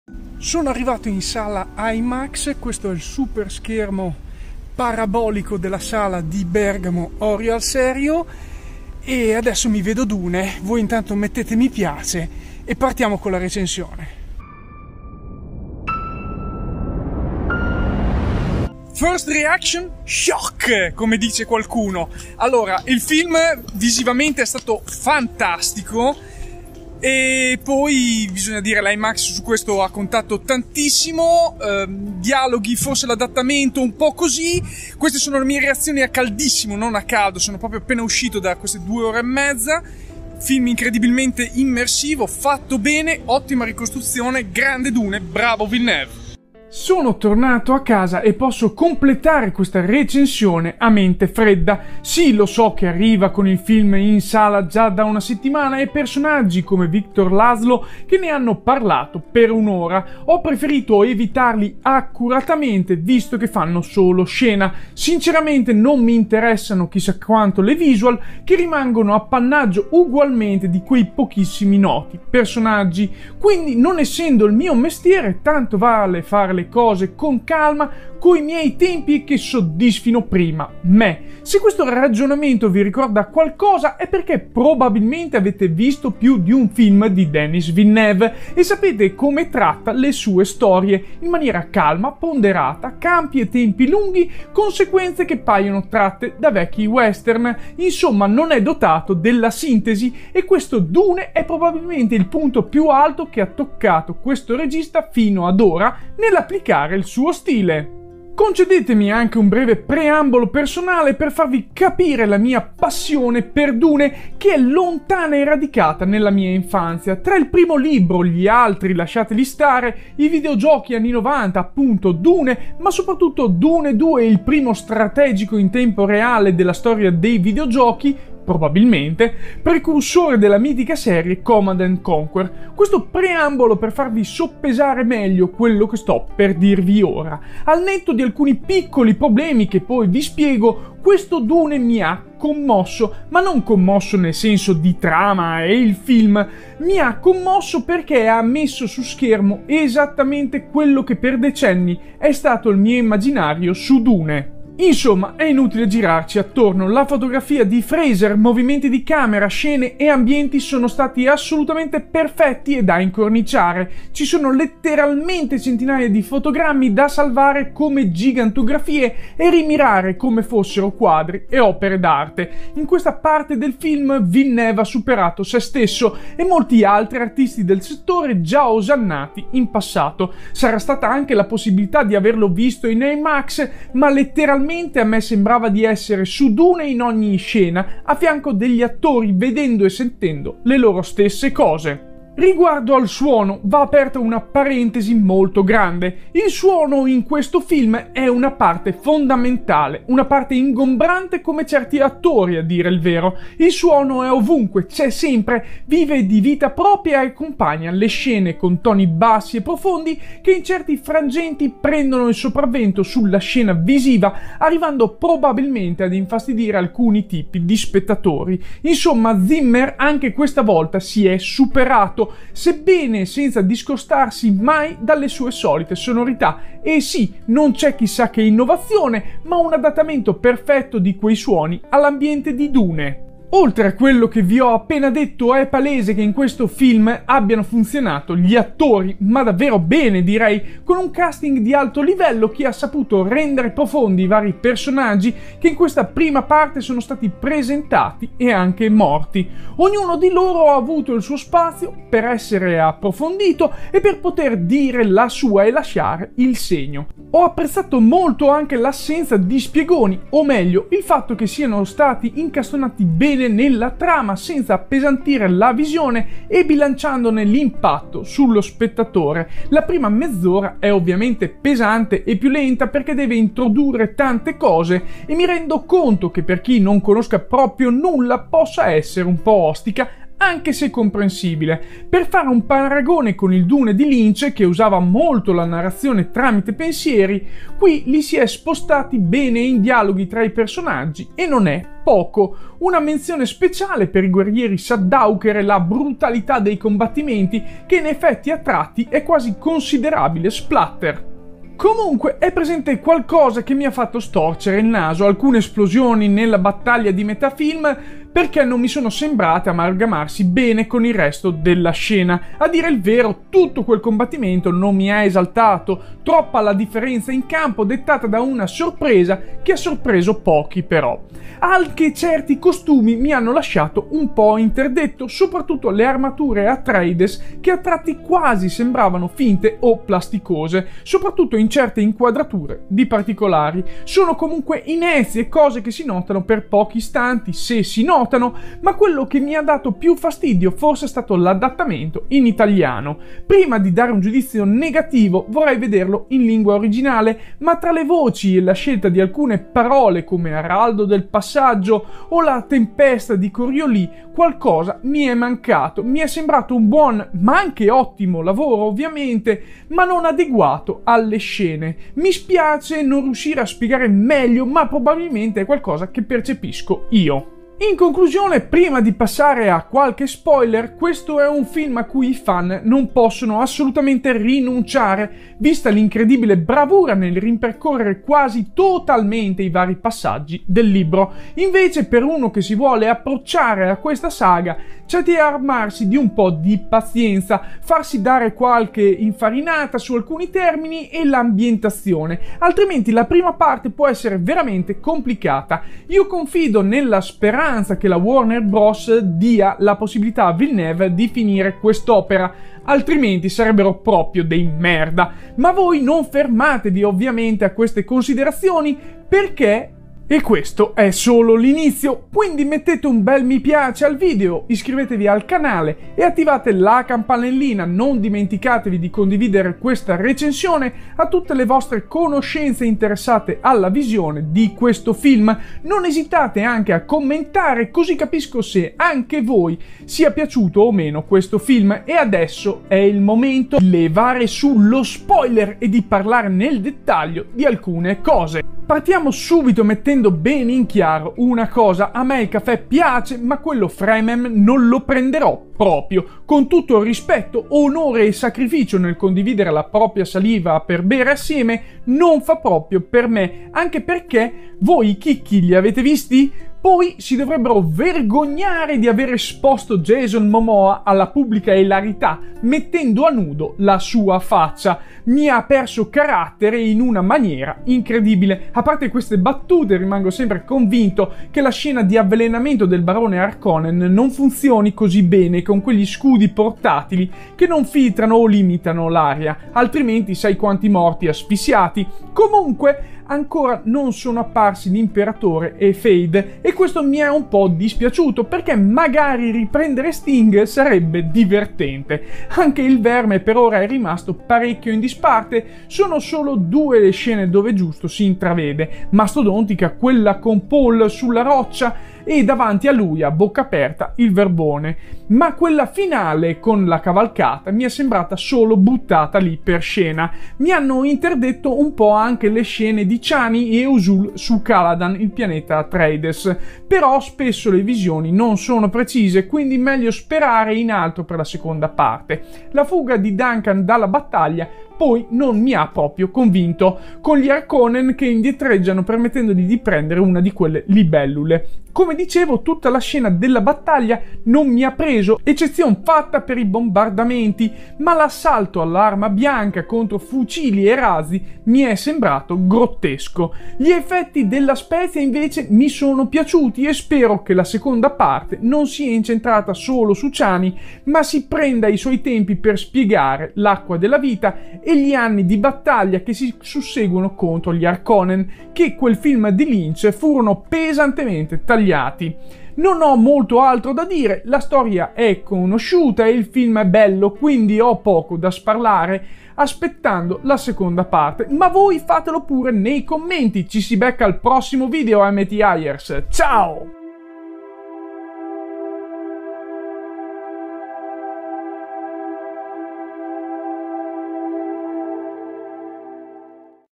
Dune di Denis Villeneuve è uno Spettacolo Recensione
| Argomenti |
0:00 In IMAX
0:18 Commento a caldo
0:46 Denis Villeneve
1:48 Piccolo preambolo
2:34 Impresa della fotografia
3:11 Il suono coinvolgente
3:53 Cosa ha funzionato
5:01 Cosa non mi è piaciuto
6:07 Perché guardare dune
7:19 Nel dettaglio